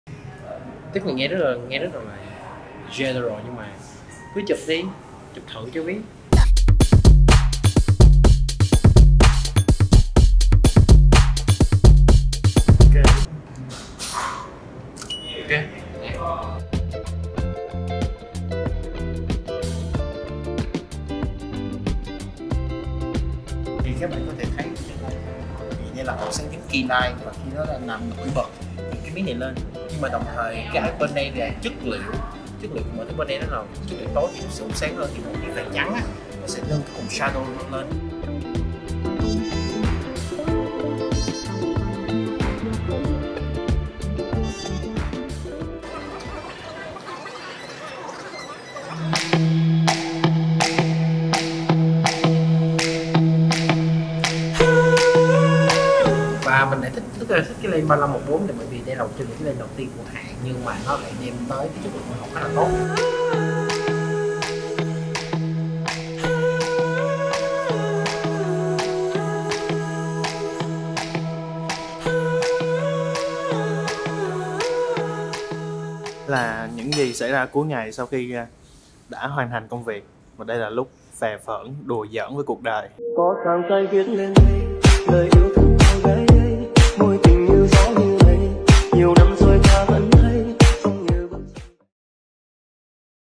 BTS Nén Eatery Shooting + tips về ánh sáng cho food photography
Đây là video BTS ngắn về buổi chụp hình cho Nén Eatery.
Trong video mình có chia sẻ vài tips về ánh sáng, cụ thể là về fill light sẽ giúp cải thiện cho shadow của bức hình, tối ưu khi về hậu kì.
Special thanks to Phương aka Jayden đã quay video này. Theo dõi Channel của Phương tại: